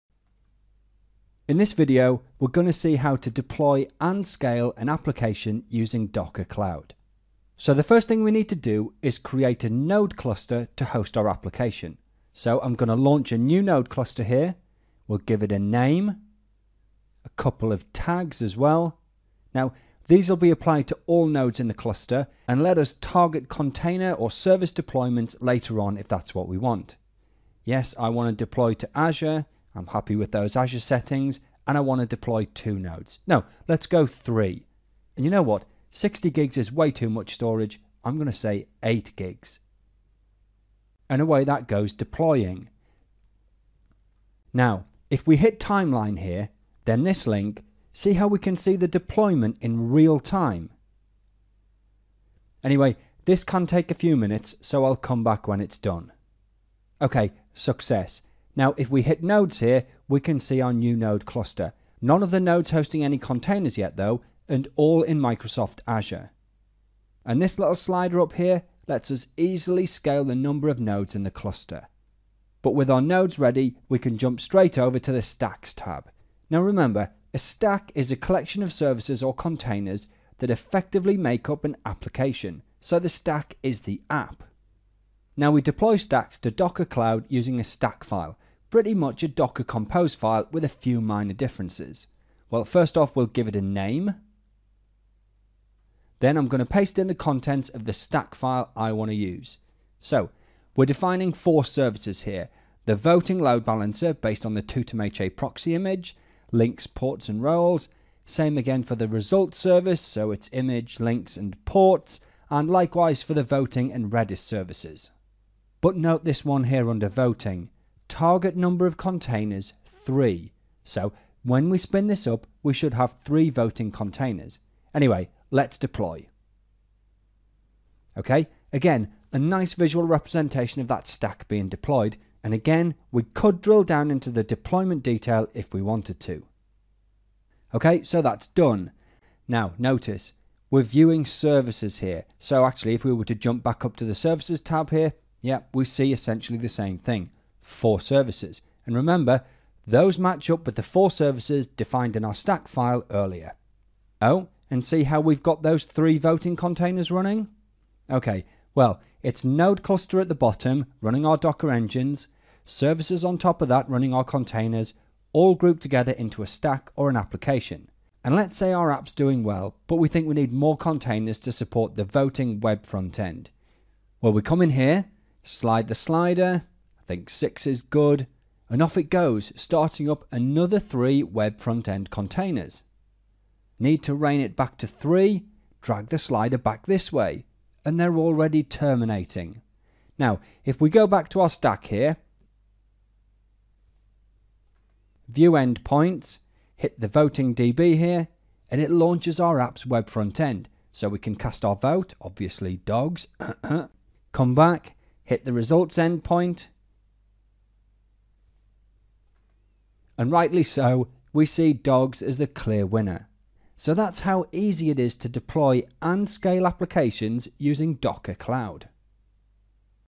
Docker Cloud - Deploy and Scale Apps
Watch this demo to see how to provision a cluster from Docker Cloud then deploy and scale a multi-container app all from a streamlined point and click UI.